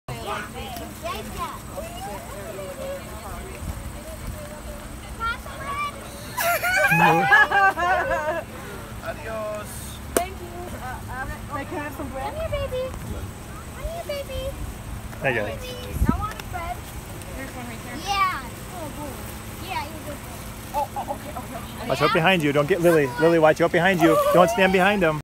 South Dakota Donkeys 2
Feeding the donkeys in Custer State Park.  SD